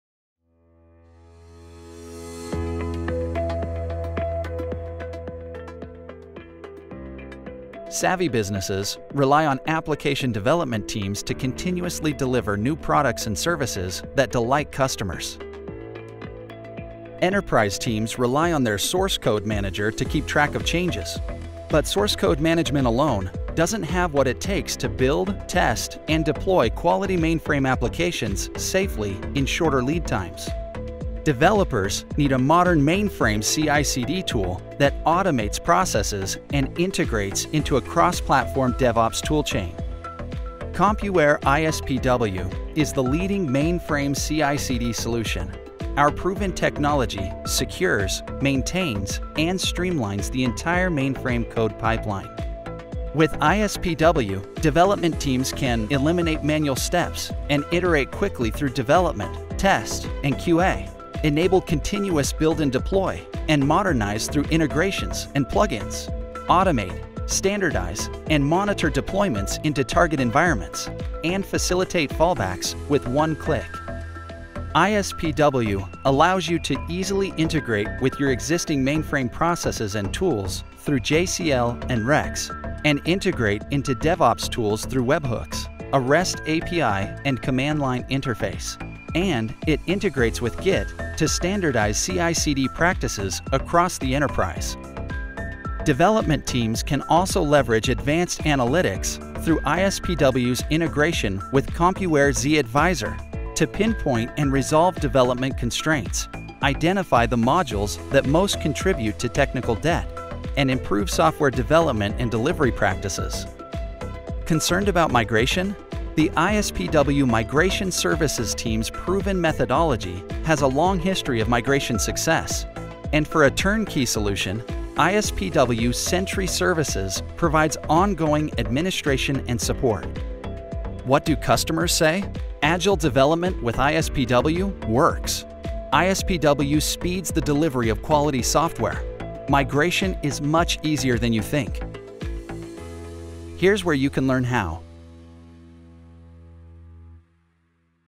ISPW: The Whole Package
It wasn’t that long ago we used phones just to make calls. Today, phones have revolutionized our lives. We have virtually everything at our fingertips in one multipurpose gadget.

Compuware ISPW is another multipurpose tool that is revolutionizing the software development experience. As the industry’s leading mainframe CI/CD solution, ISPW:
- Manages source code
- Can be leveraged to build, test and deploy quality mainframe applications safely
- Automates processes and integrates into a DevOps toolchain
- Integrates with Compuware zAdviser for insight into development bottlenecks and modules that contribute to technical debt

ISPW’s differentiators are copious. Watch this short video to see what ISPW does, that no other development tool can. See why ISPW is the whole package.